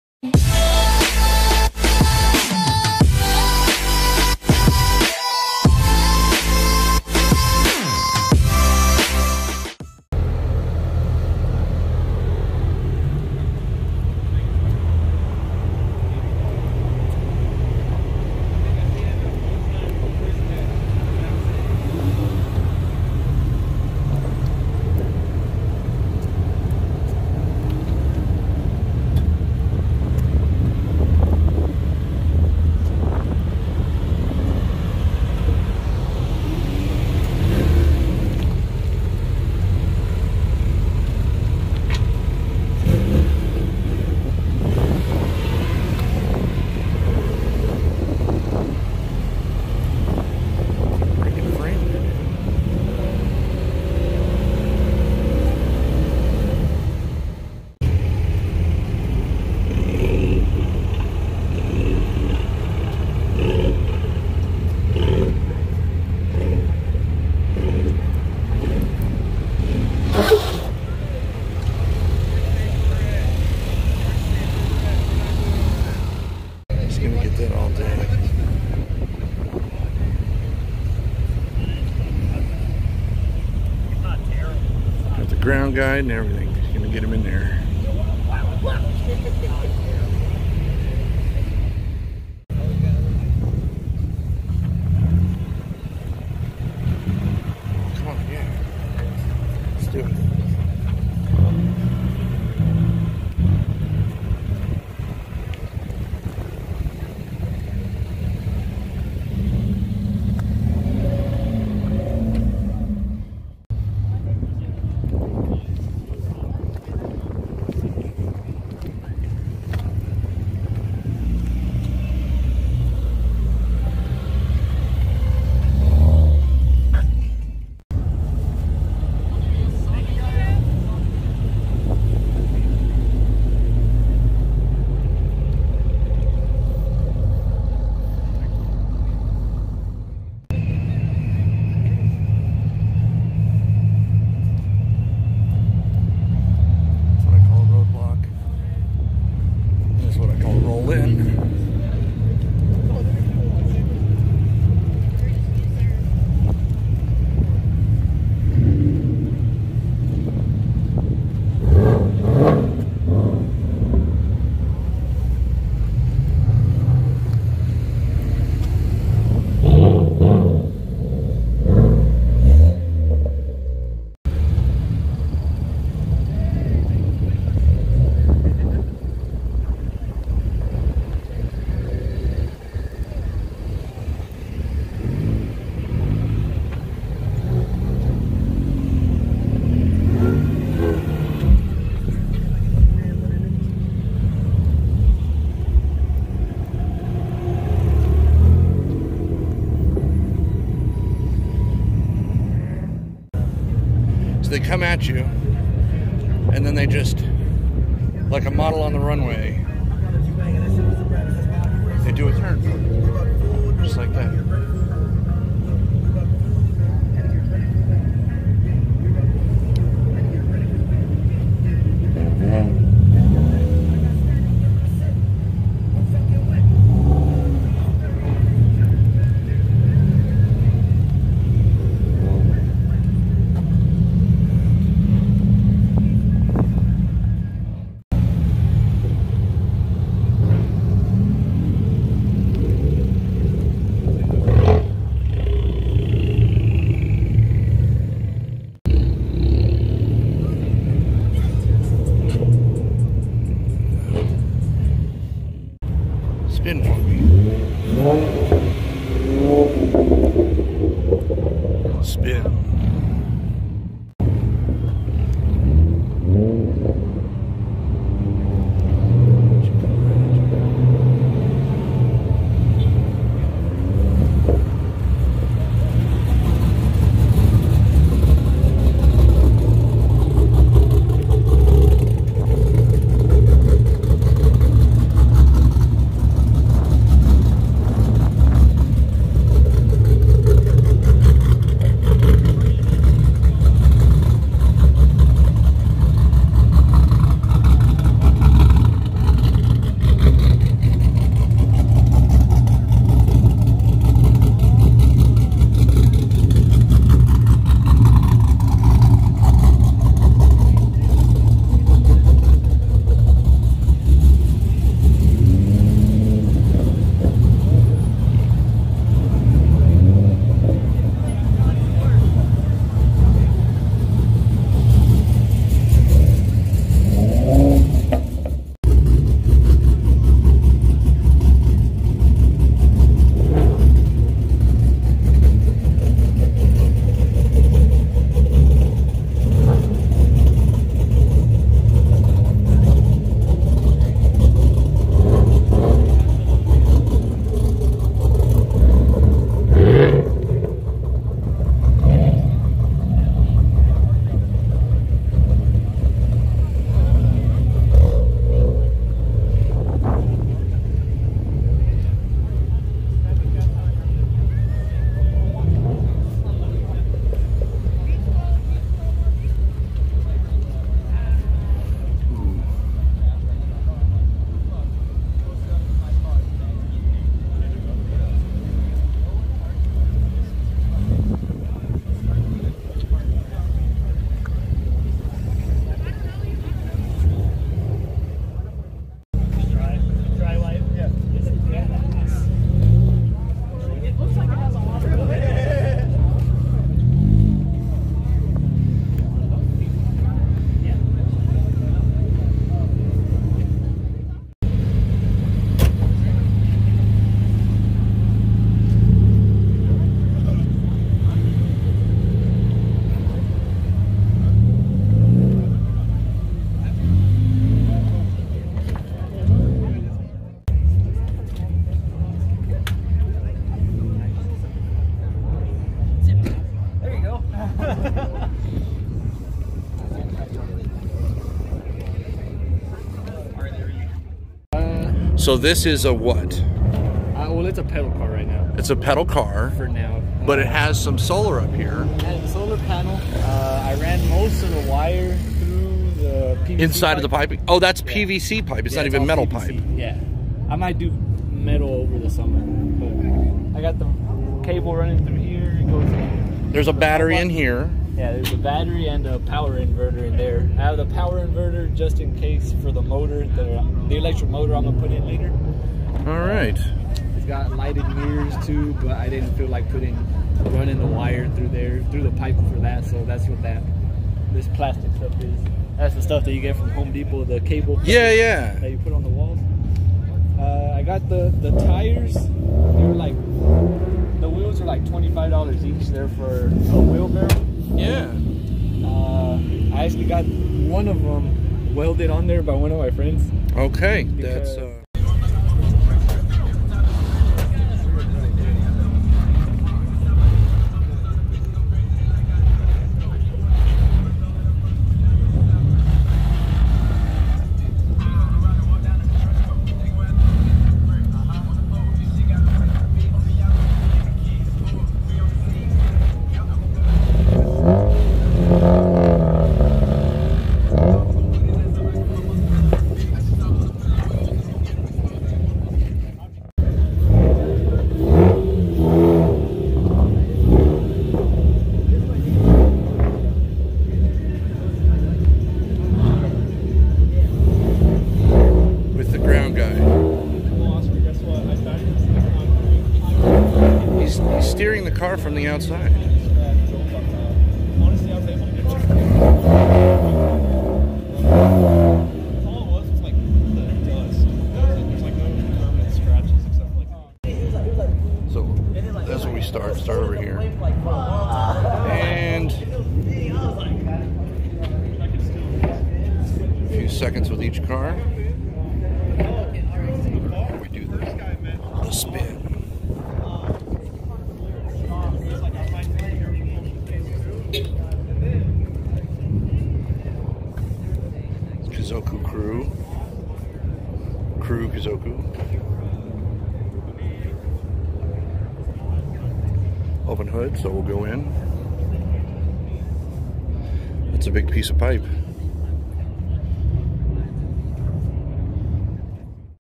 Mavs + Motors 3/6/20 Part I
The first portion of my Video coverage from Mavs + Motors on March 6th 2020 at UTA West Campus parking Garage.